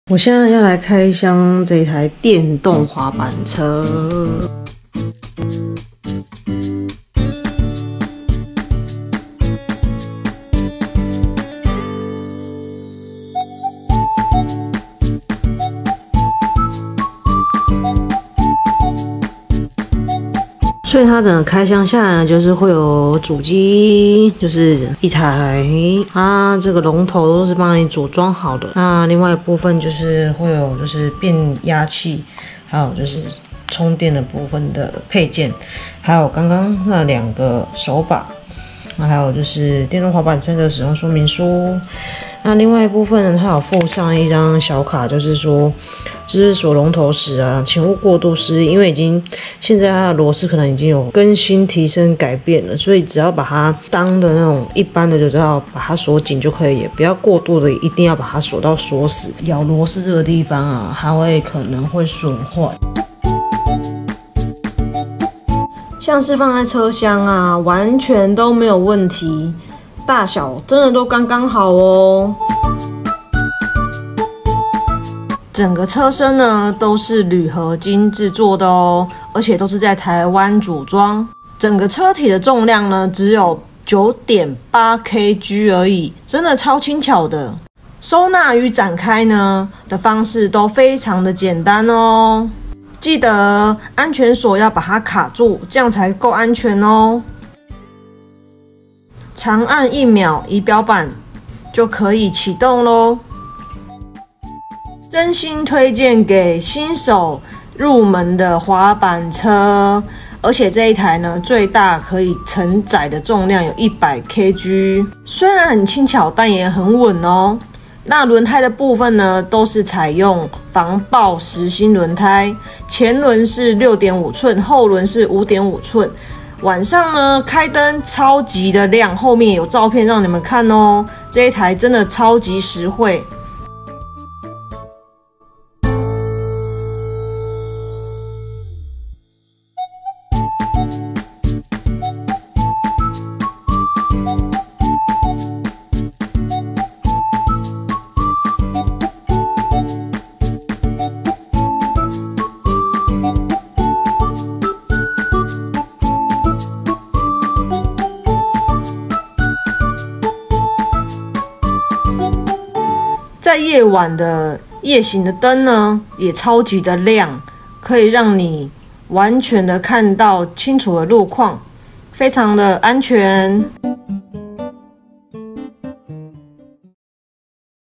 想入手電動滑板車嗎？Waymax | Lite-1 電動滑板車，新手入門款推薦！車身重量只有9.8 kg我自己女生一手都扛的起來。
電動滑板車真的近期好流行！
起初我對於電動滑板車會感覺真的有這麼厲害嗎？
真的有這麼穩嗎？蓄電力夠嗎？其實產生蠻多疑問的！
身為新手的我選擇Waymax Lite-1電動滑板車
在官網也是新手入門款哦在價格上也很甜
基本上入手荷包也不會痛
真正用過Waymax Lite-1電動滑板後就會真正愛上他（笑）！
車身材質使用鋁合金，車身重量只有9.8 kg我自己女生一手都扛的起來超輕，而且整台車都是台灣組裝喔，而且都是防爆實心胎所以輪胎不需要自己充氣，晚上騎也沒問題有超亮的車燈前燈+後輪剎車亮燈，晚上騎乘安全度也提升。
Waymax｜Lite-1 電動滑板車這邊買

✔我的其他平台⇩
相機：Sony A73